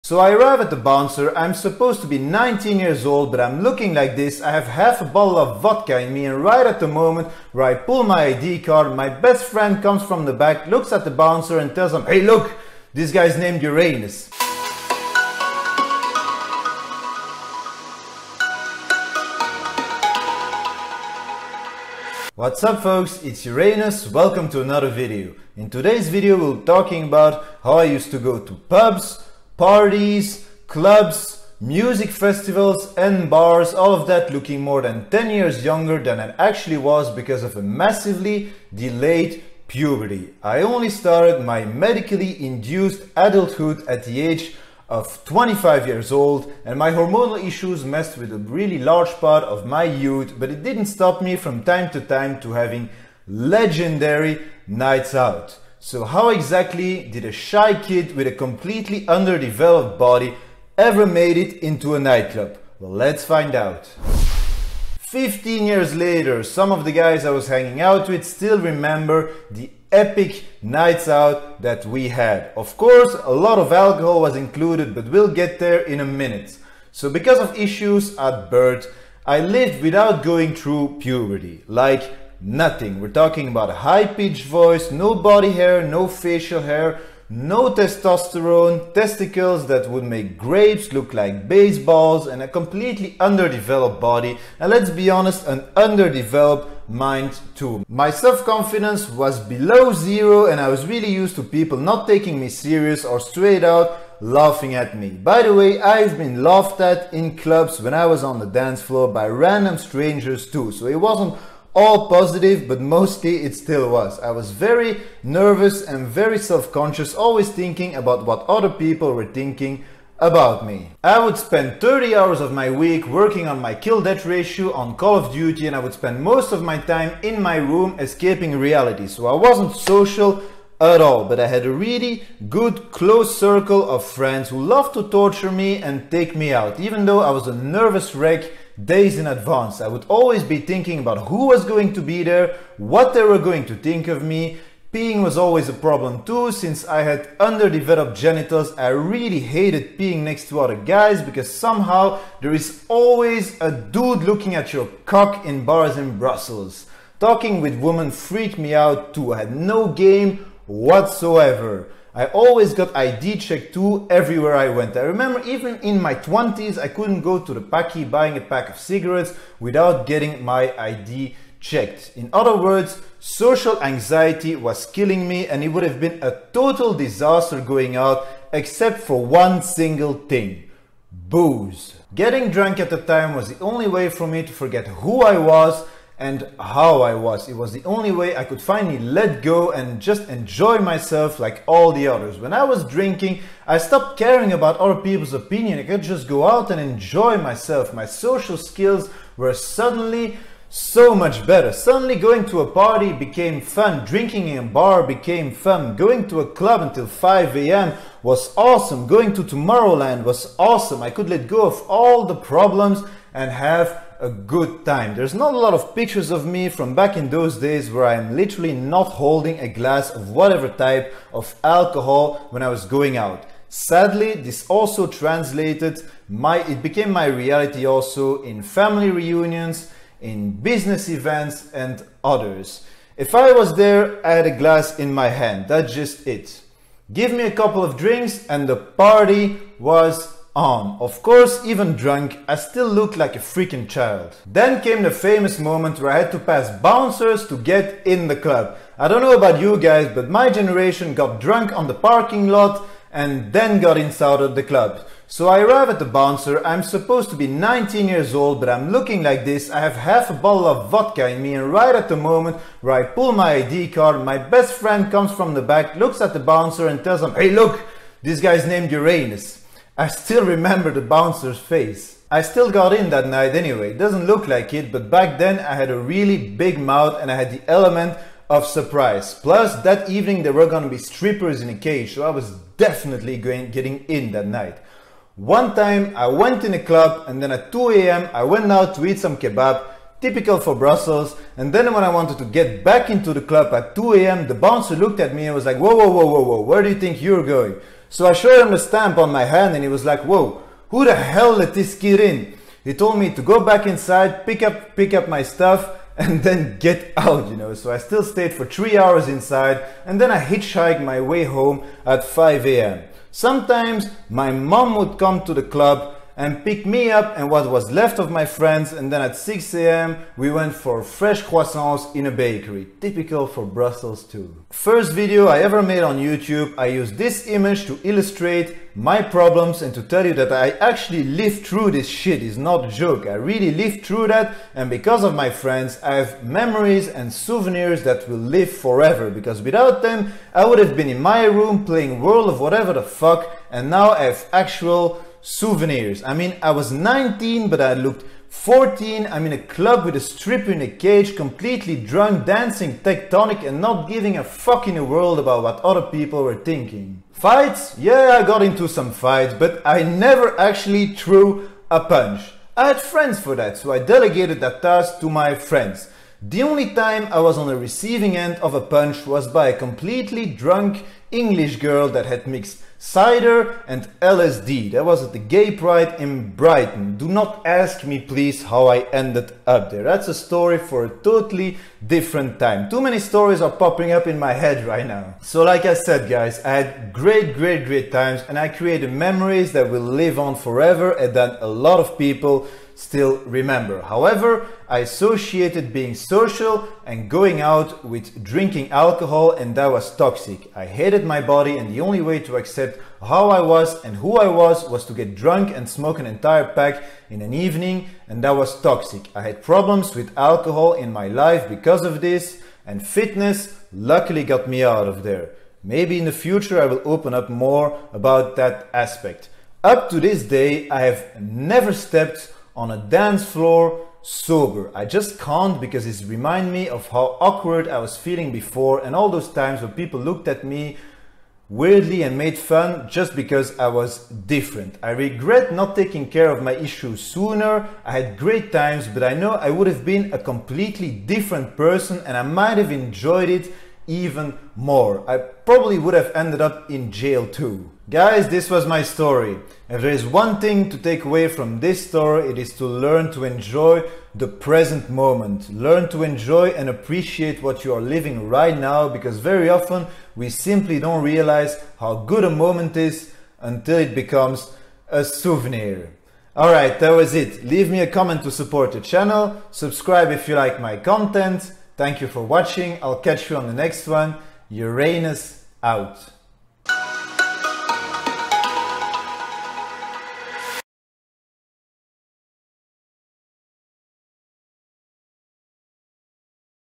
I went to nightclubs & parties before even reaching puberty
Because of extremely delayed puberty I lived the better part of my youth with a completely undeveloped body and mind. I suffered form social anxiety and use to game 30+ hours per week. Even though I was a completely different person and I was bullied every day, it didn’t stop me from going out and having epic night outs in bars, clubs, pubs and parties.